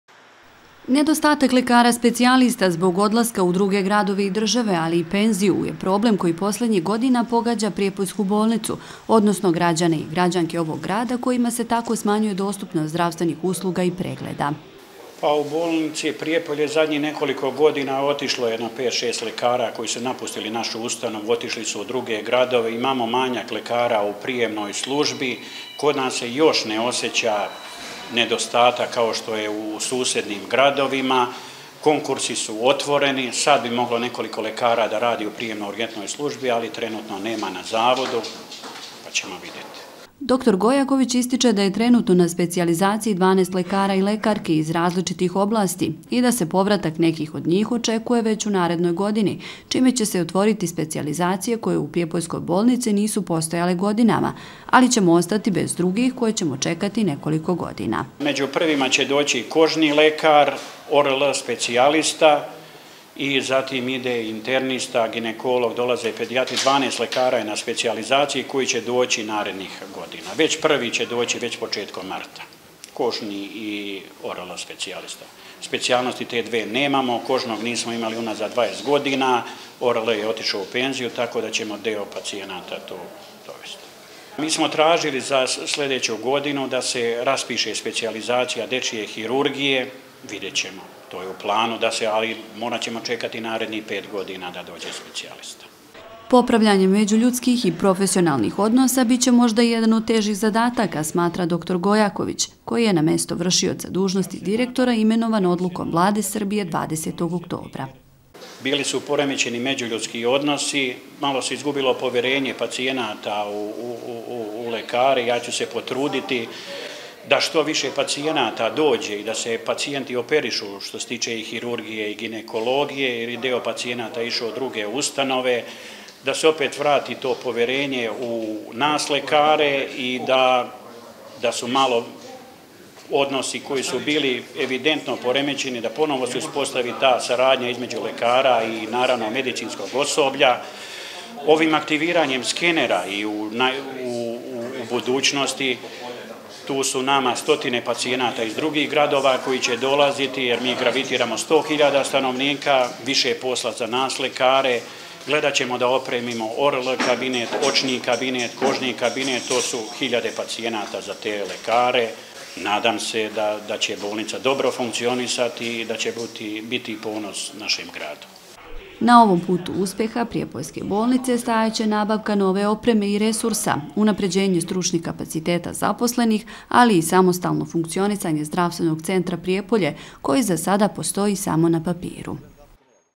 Dr Branko Gojaković o funkcionisanju Opšte bolnice Prijepolje
Rešavanje problema nedostatka lekara specijalista, otvaranje novih kabineta, popravljanje narušenih međuljudskih odnosa i vraćanje poverenja građana i građanki Prijepolja u rad zaposlenih u Opštoj bolnici Prijepolje – biće zadaci novog vršioca dužnosti direktora ove ustanove dr Branka Gojakovića.